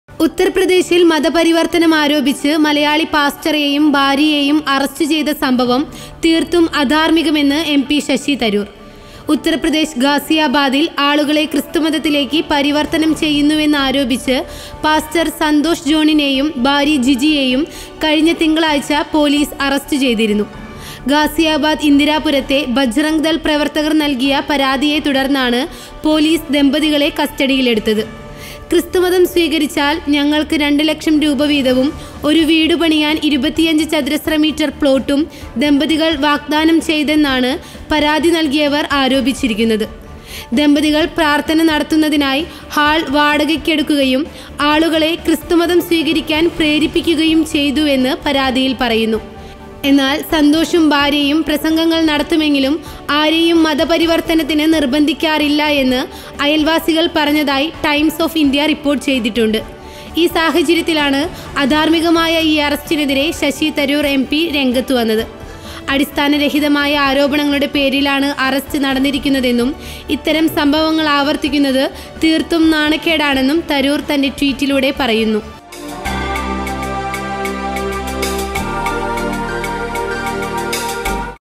പ്രതികരണവുമായി ശശി തരൂർ|CHRISTIAN PASTOR ARRESTED|UTTARPRADESH|CHURCH|SHASHI THAROOR|GOODNESS TV
മതപരിവർത്തനം ആരോപിച്ച് മലയാളി ദമ്പതികളുടെ അറസ്റ്റ്
പ്രതികരണവുമായി ശശി തരൂർ